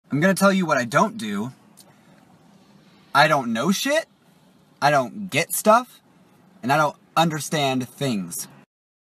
i dont know shit
animation of my oc aiden

this video is not intended for or targeting kids. If you are younger than thirteen, please refrain from watching this video